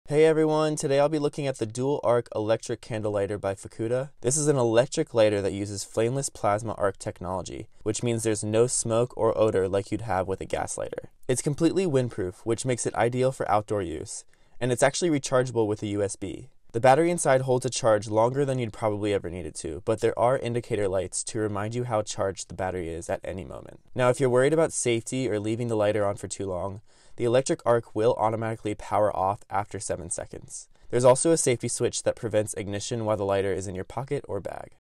Fukkuda Electric Windproof Flameless Lighter
Fukkuda Electric Plasma Ark Lighter For Candles

Dual Arc Electric Candle Lighter Rechargeable USB Lighter Plasma Arc Lighters for Candle (Rose Gold)

I received this product for free, but any opinion expressed is strictly my own!